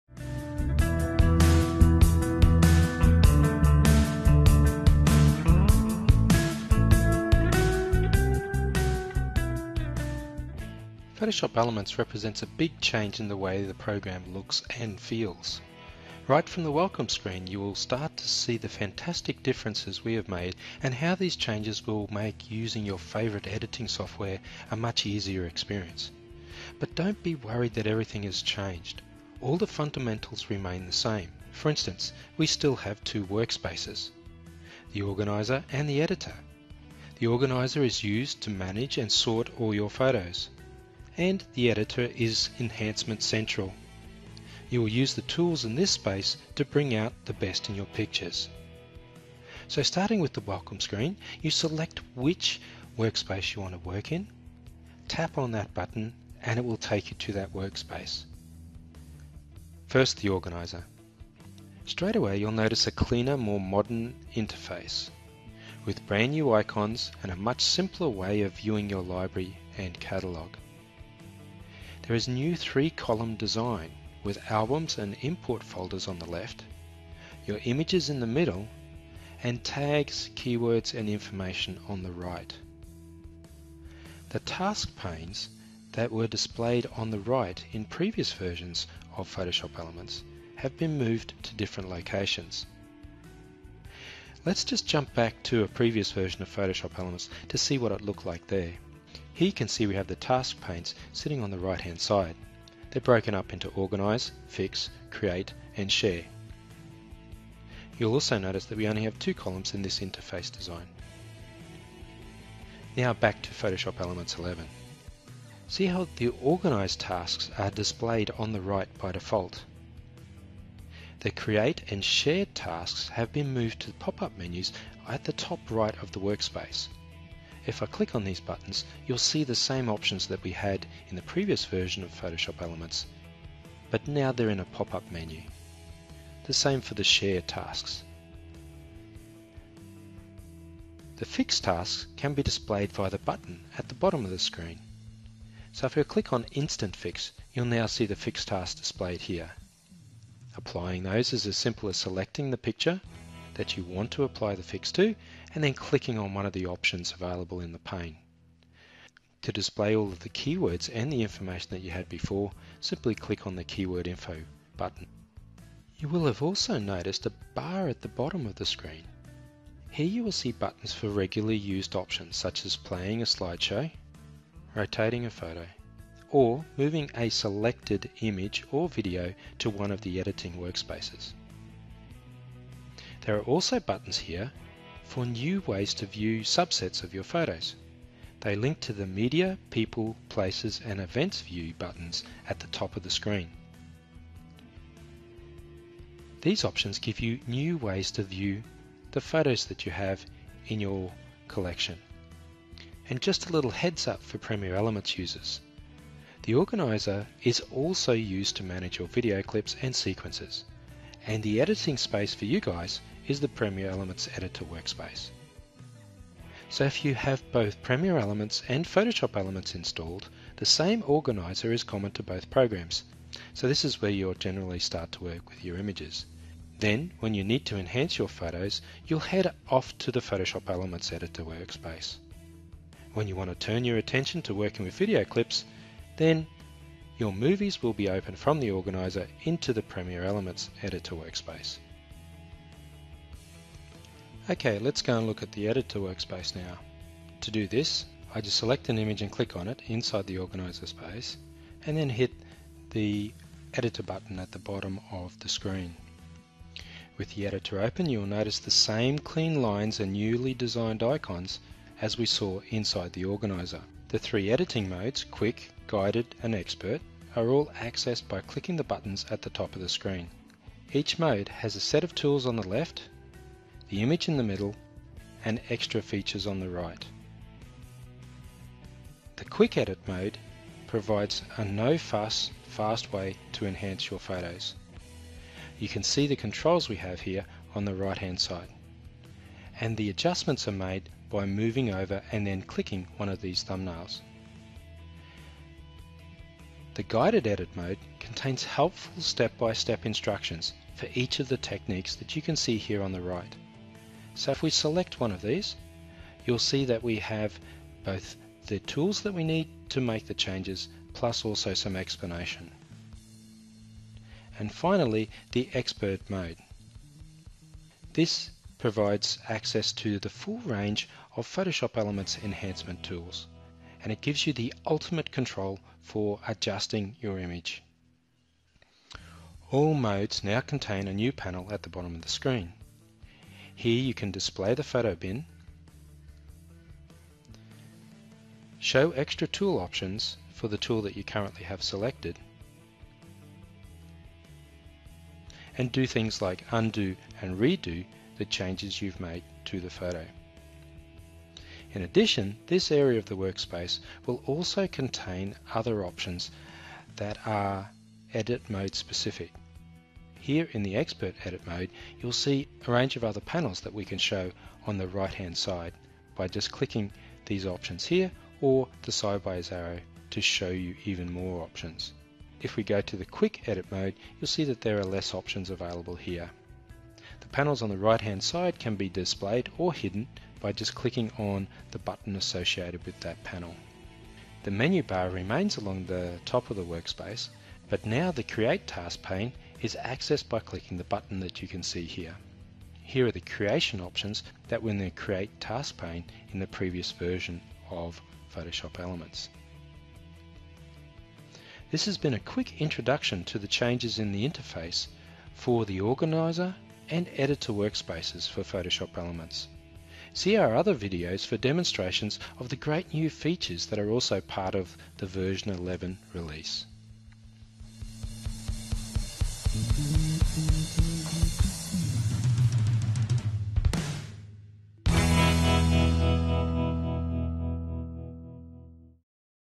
The New Photoshop Elements 11 User Interface
Take a tour of the new, modern interface in Photoshop Elements 11. Explore the three-column design of the updated Organizer, learn about the options in the Task bar, and see how you can easily view subsets of photos by People, Places, Events, and even Media. Also check out new Quick, Guided, and Expert editing modes, which enable you to edit photos the way that works best for you.